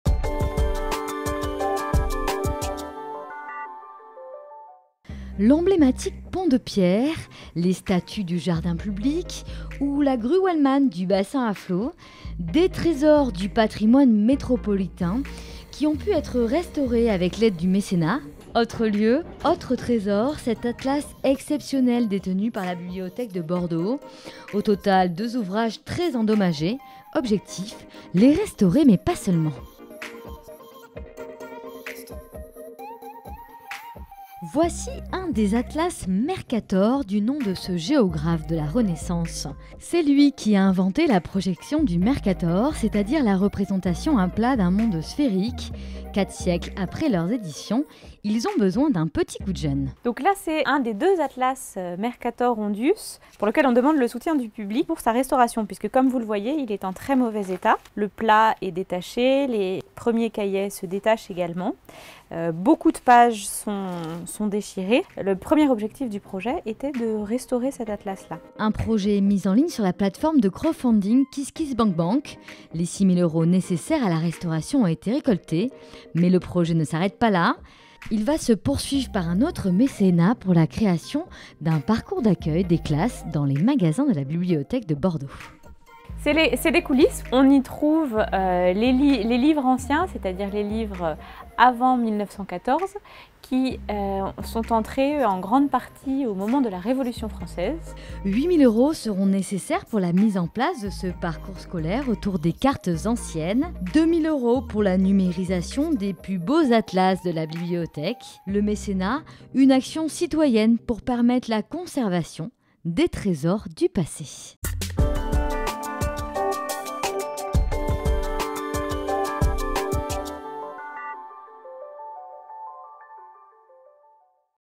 Bordeaux Métropole, Pourquoi, Comment ? - Bordeaux veut sauver les atlas Mercator et Hondius
Bordeaux métropole, Bordeaux veut sauver les atlas Mercator et Hondius.